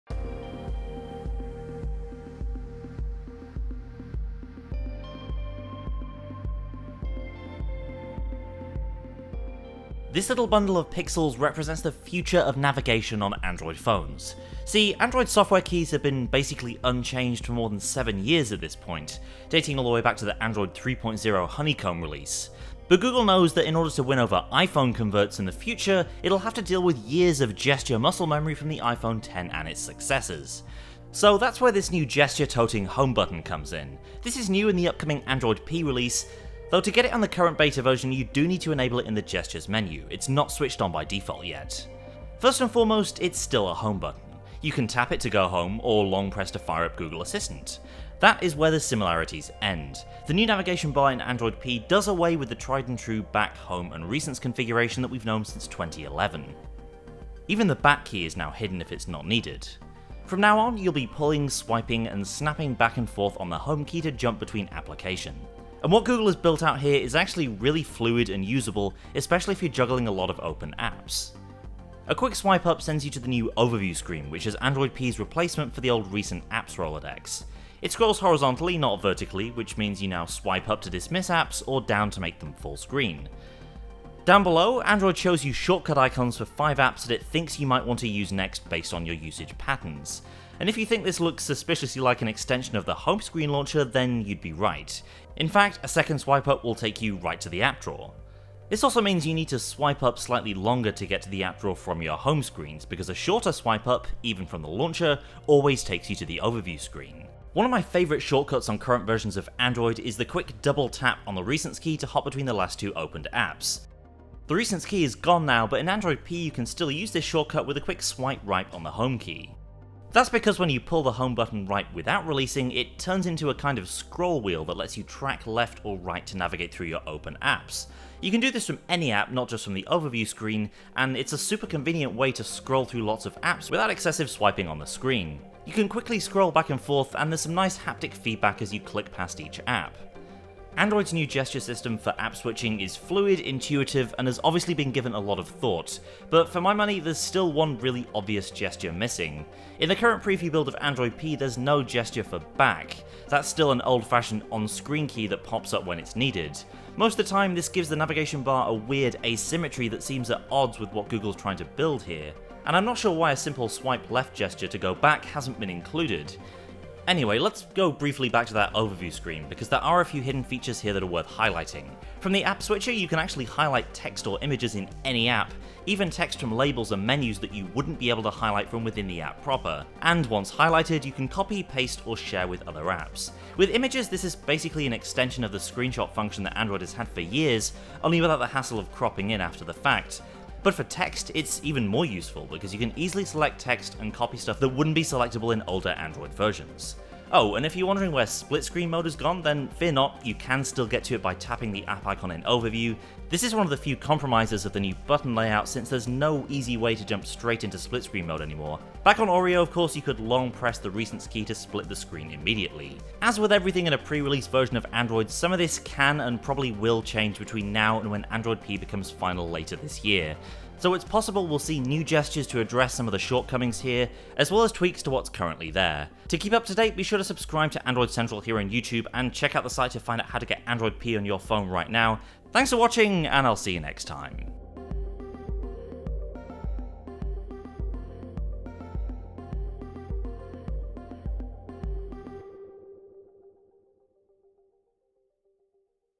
Android P Gestures Explained! [Android 9.0 Beta]
Android P has a wild new gesture navigation system. Here's how it's different from the button-based system we've been using since Android 3.0 -- and everything it can do!